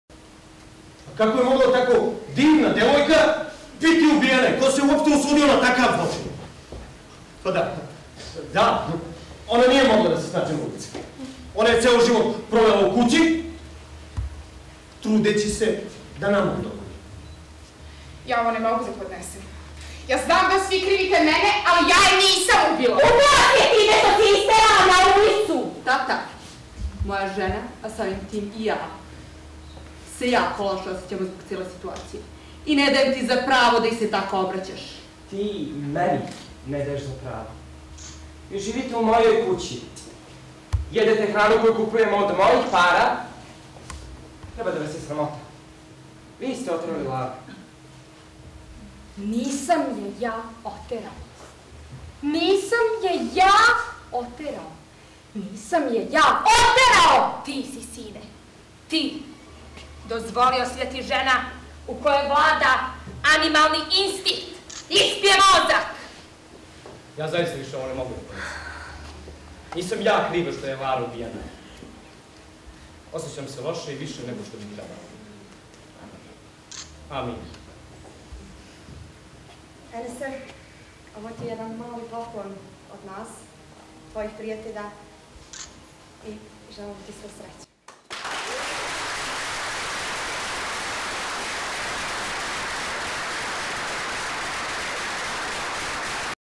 Promocija drame NEKO JE UBIO LARU Enesa Sijarica deo II
Drama Enesa Sijarića, "Neko je ubio Laru" je objavljena u okviru edicije 'Principium' koja obuhvata izdanja Narodne biblioteke "Dositej Obradović" Novi Pazar, nagrađena na Konkursu za objavljivanje prve knjige, a čiji je cilj afirmisanje zavičajnih talenata i osnaživanje lokalne književne scene, koja će predstavljati naš grad u zemlji, regionu i svetu.Drama Enesa Sijarića Neko je ubio Laru, po rečima žirija, je originalno delo pogodno za izvođenje i nosi sve odlike savremene evropske drame. Tekst pruža mogućnost bliske komunikacije sa publikom, sadrži simboličan značenjski sloj i ne bi izgubio ništa od aktuelnosti i vrednosti prikazivanjem u drugim kulturama i izvođenjem na drugim jezicima.

Na promociji su govorili Melida Rebronja, profesorica srpskog jezika u novopazarskoj Gimnaziji, Radoman Čečović, recenzent drame i sam autor. Melida Rebronja je izrazila zadovoljstvo što je talenat njenog nekadašnjeg đaka prepoznat, koji je već duže vreme poznat novopazarskoj publici po svojim režiserskim i glumačkim sposobnostima, budući da se drama, čiji je on autor, igrala više puta u zemlji i inostranstvu u izvođenju Dramske trupe Joj evo ih ovi, čiji je osnivač i koordinator sam Sijarić.

Recenzent knjige, crnogorski pesnik Radoman Čečović je pročitao svoju recenziju Sijarićevog djela u kojoj ističe kako tema koju autor prikazuje nije od velikih ovovjekovnih nedoumica u čijem je centru čovjek, nesrećan i prolazan, već je pre svega pokušaj razračunavanja sa samim konceptom drame kao forme i ispitivanje sredstava koja je književna tradicija ostavila u nasleđe. Enes Sijarić posjeduje sve one signale kojima se nagoveštava rađanje kvalitetnog dramskog pisca, koji je prije svega čitalac, a tek onda stvaralac, zaključio je Čečović.

Autor se zahvalio gostima i prijateljima na podršci ispričavši kako su izgledali njegovi prvi koraci u svetu glume i režije i objasnivši zašto se kroz čitavu dramu provlači motiv šaha, kao inteligentne igre koja simbolizuje igru života, a čija igra se konstantno odvija u porodici Hauštufirel. Dramska trupa Joj evo ih ovi je na samom kraju večeri odigrala odlomak iz drame Neko je ubio Laru, nakon čega su prisutni mogli kupiti knjigu.